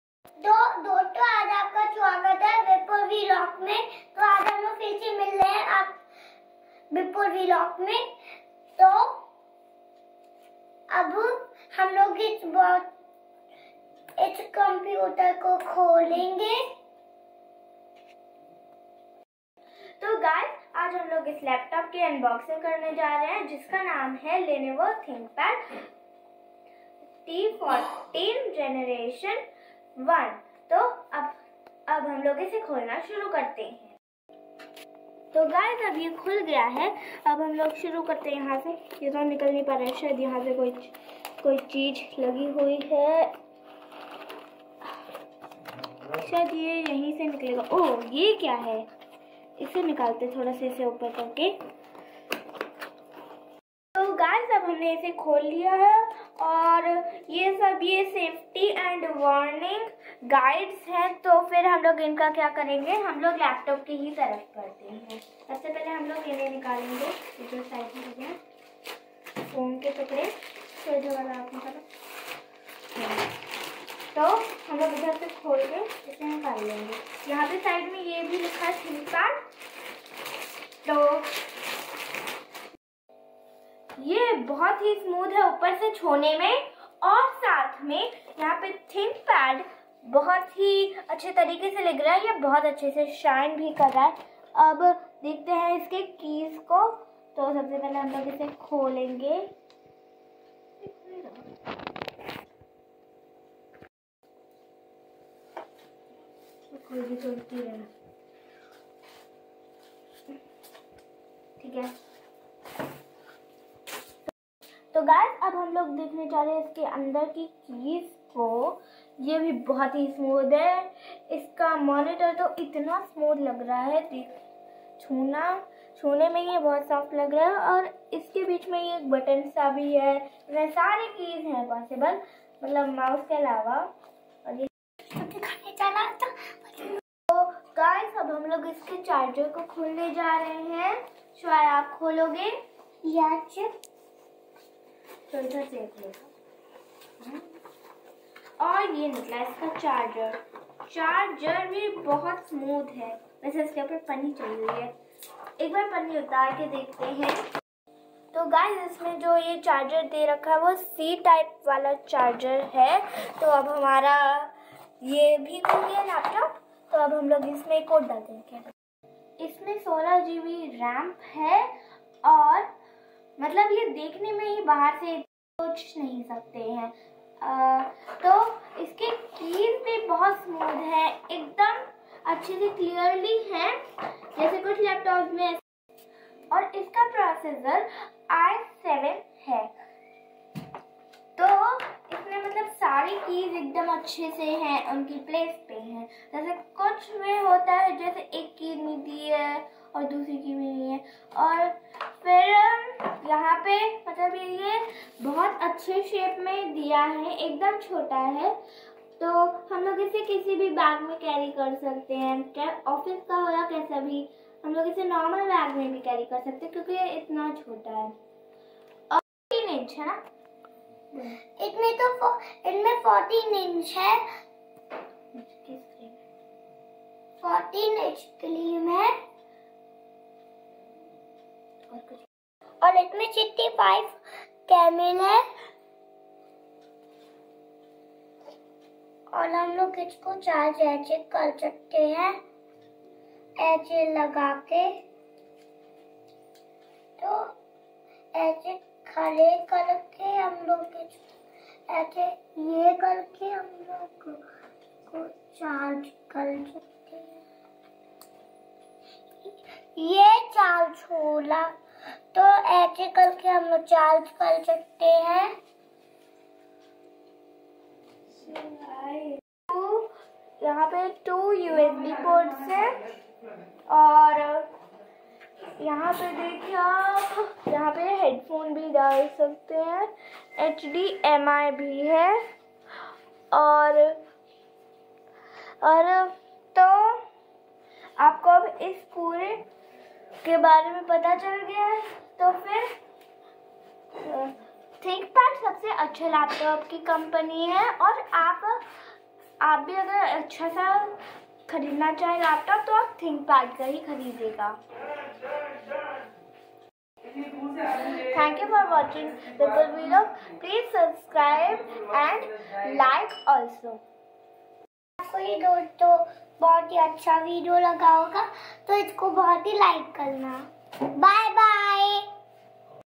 Cutest Unboxing Of my Official Laptop | Lenovo ThinkPad T14 G1
In this laptop Cutest unboxing of Lenovo ThinkPad T14 G1

My Bhanja Bhanji Misty and Shivay

for Purchase Link is here